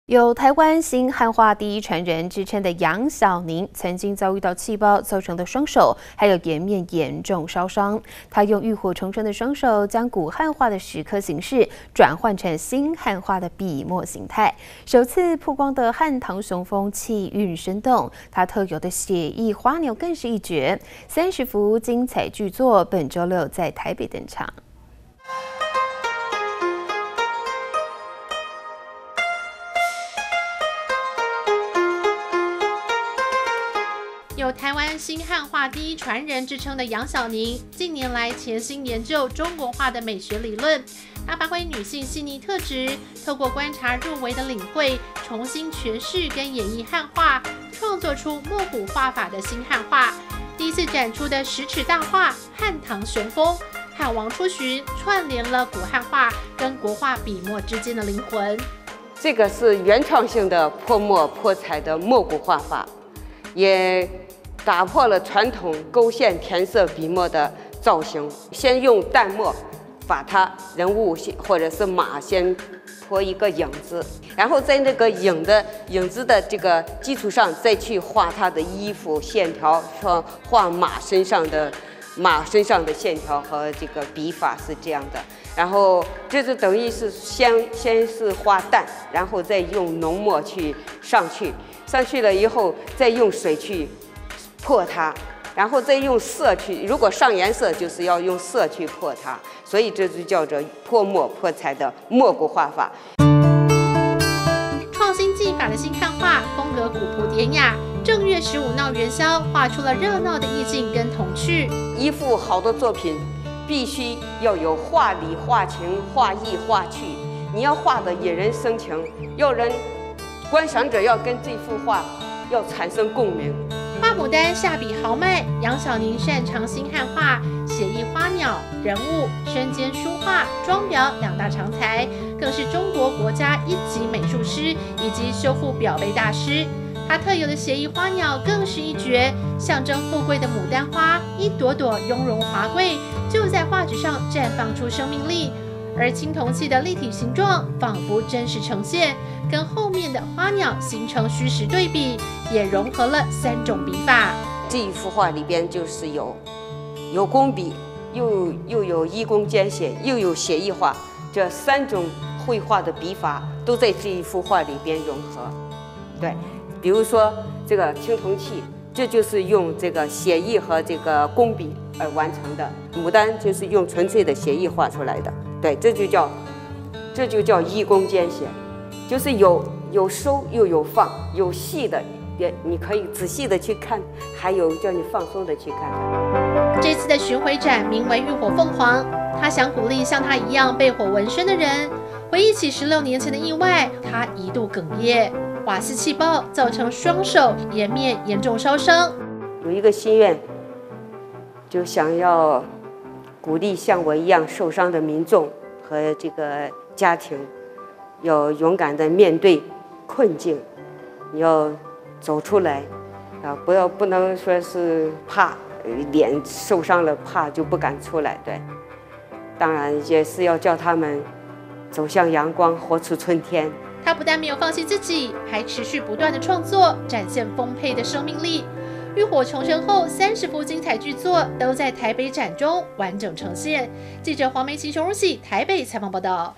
台灣新漢畫第一傳人 國畫大師"楊小寧"辦展.30幅巨作精彩亮相｜非凡財經新聞｜20220708
有「台灣新漢畫第一傳人」之稱的楊小寧曾遭遇氣爆造成雙手、顏面嚴重燒傷，她用浴火重生的雙手，將古漢畫的石刻形式轉換成新漢畫的筆墨型態，首次曝光的「漢唐雄風」氣韻生動，她特有的寫意花鳥更是一絕，30幅精彩鉅作本週六台北登場。